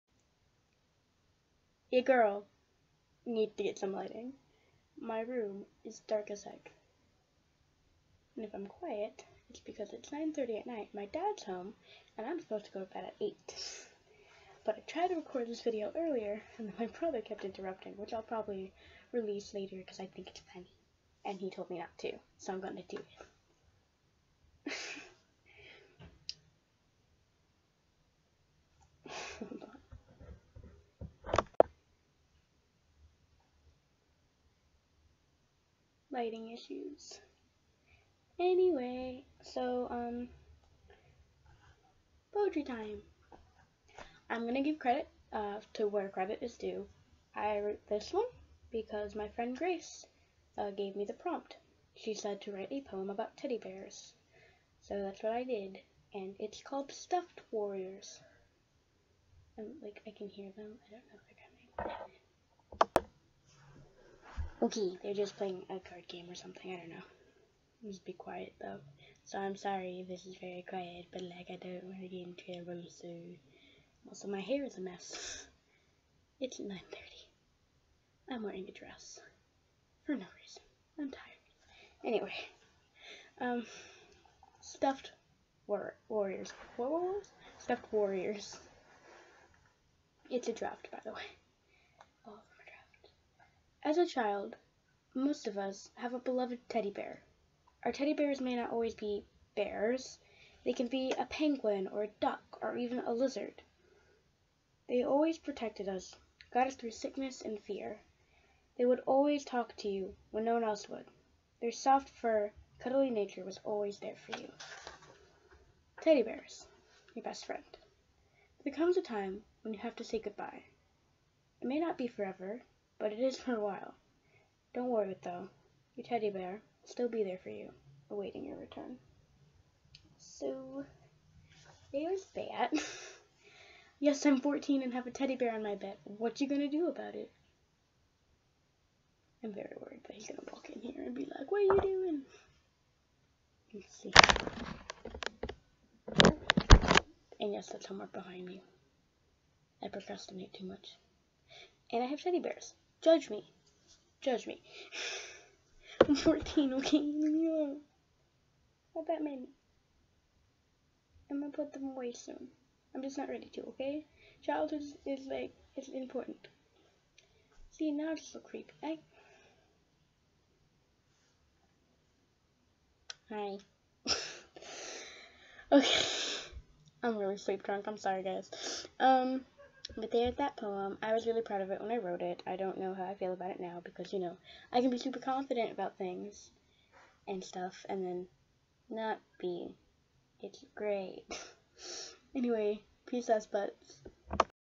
Stuffed warriors - a poem
leaving childhood behind is hard...
Lines
As a child
most of  us
have a beloved
teddy bear.
Our teddy bears may not always be
bears.
They can be
a penguin,
a duck,
or even a lizard.

They always
protected us.
Got us through
sickness and fear.
They would always talk to you
when no one else would.
There soft fur and cuddly nature
was always there for you.

Teddy bears,
your best friend.

But there comes a time
when you have to say
goodbye,
it may not be forever
but it is for a while.
Don't worry though,
your teddy bear will still
be there for you
awaiting your return.